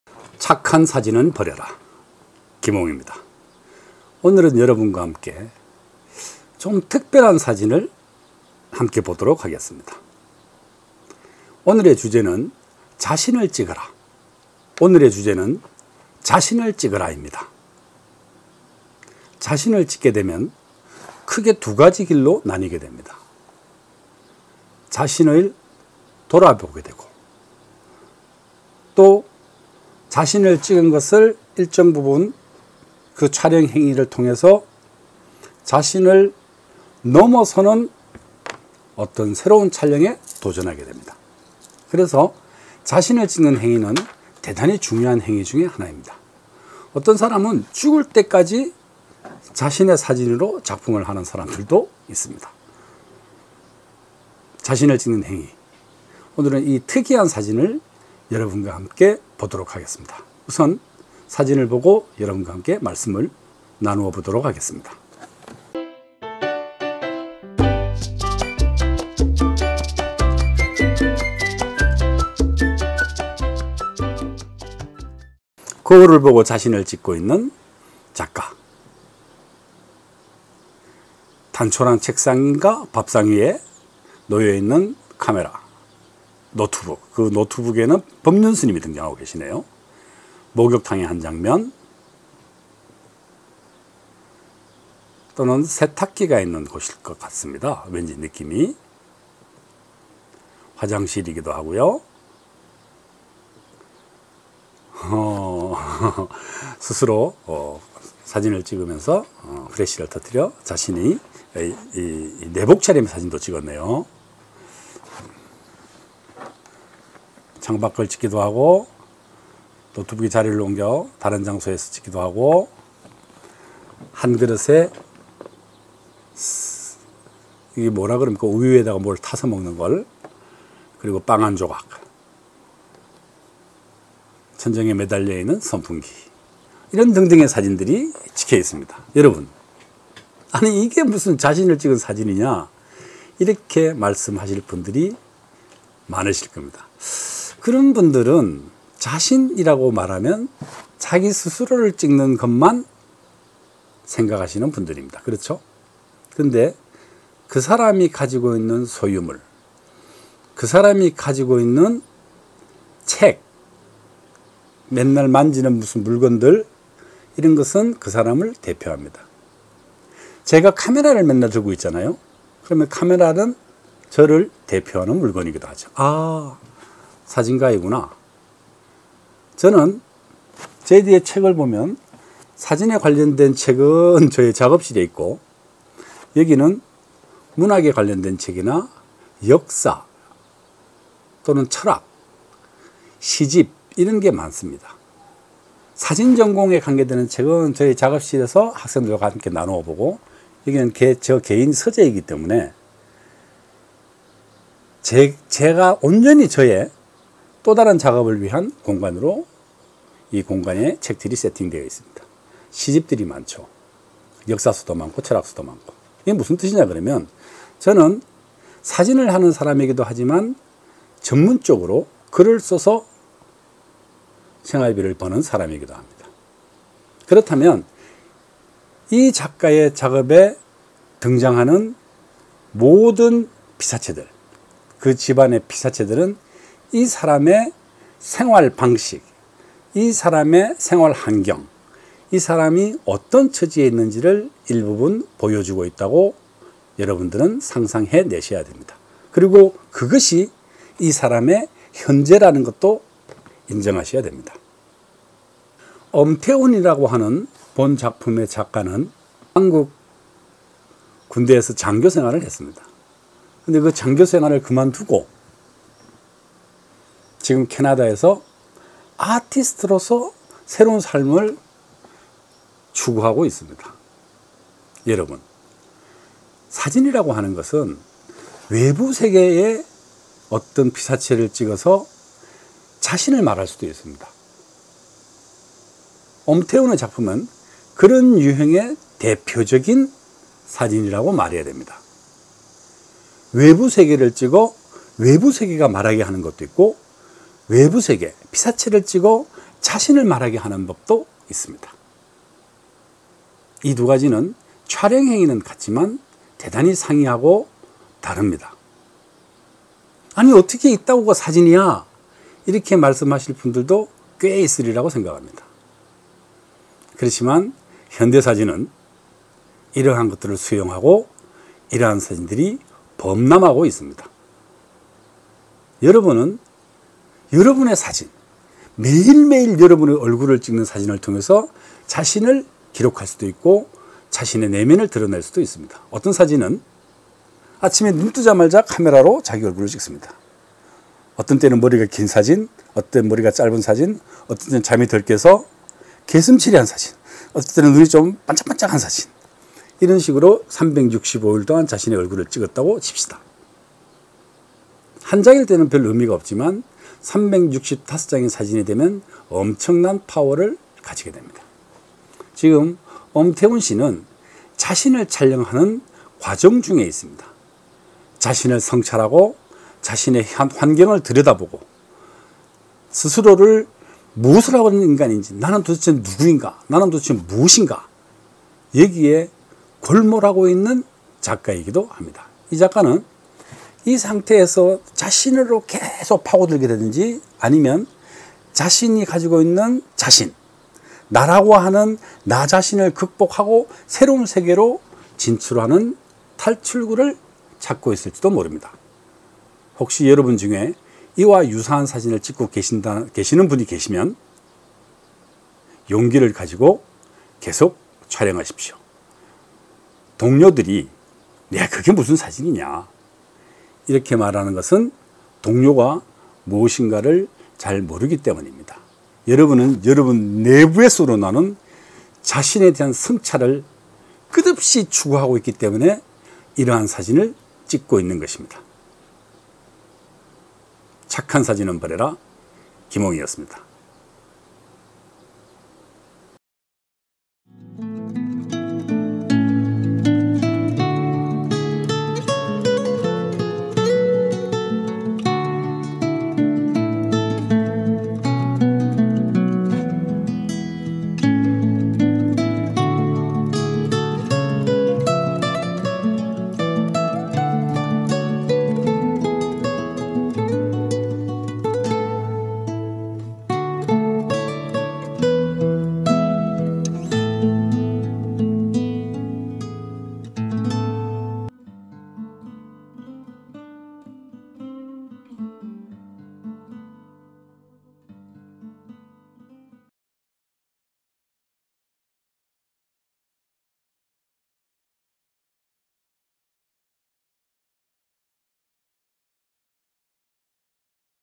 3-5.나 자신을 찍어라(엄태훈 작가)/착한 사진은 버려라
자신을 찍는 것은 참 자아의 탐구이자 자기 극복의 열쇠다.